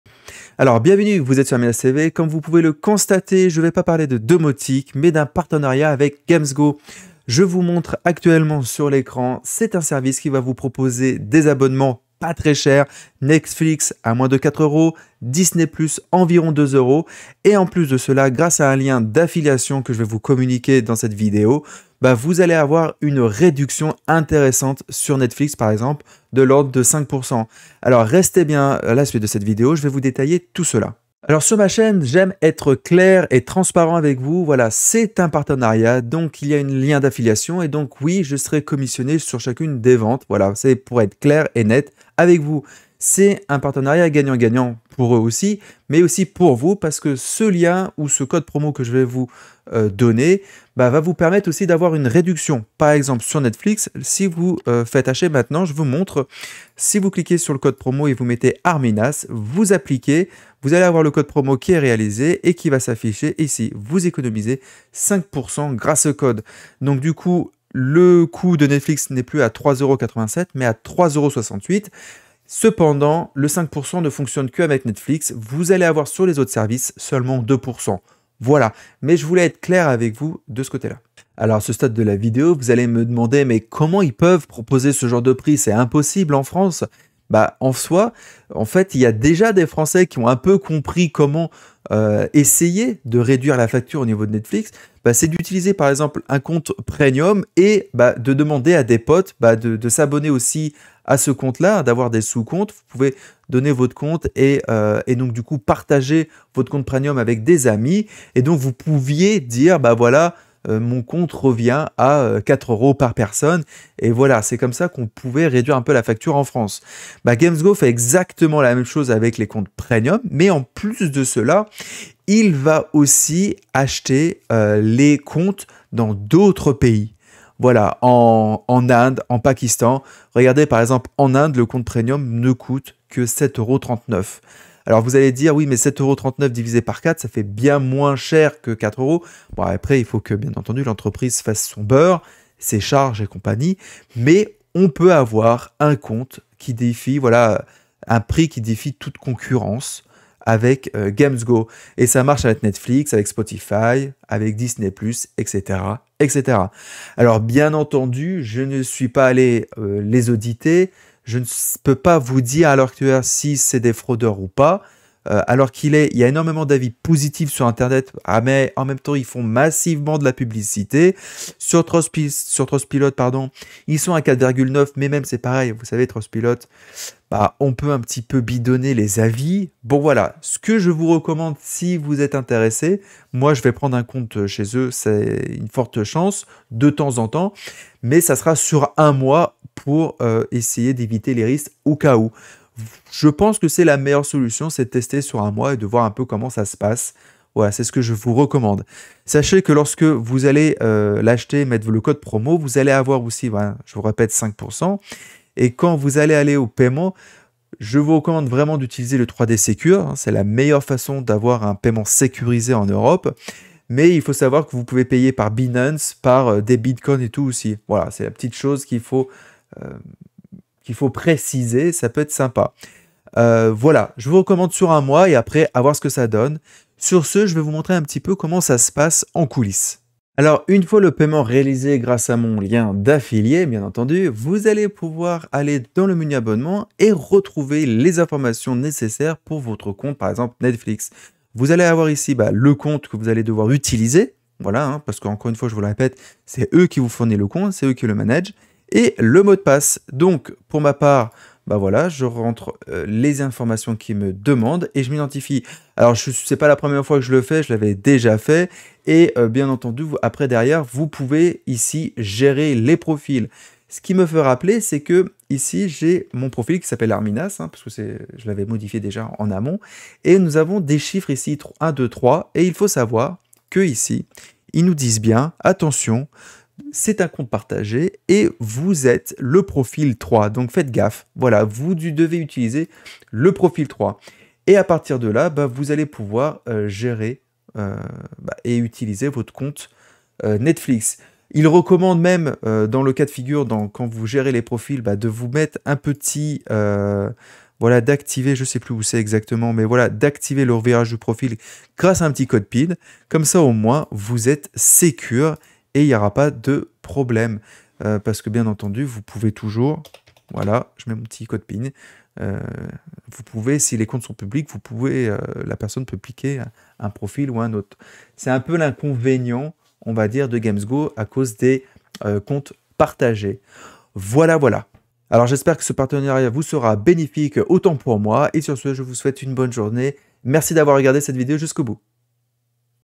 Gamsgo Netflix, Disney+, Paramount, Spotify ... pour des prix 3 fois moins cher !!!
Avec GamsGo, vous allez pouvoir payer netflix bien moins cher que les prix habituels en France.

Avec le lien, vous aurez 5% sur Netflix et 2% sur les offres offres !!!

🙏🙏   Pour soutenir la chaine c'est juste en dessous   🙏🙏

Miniature par Canva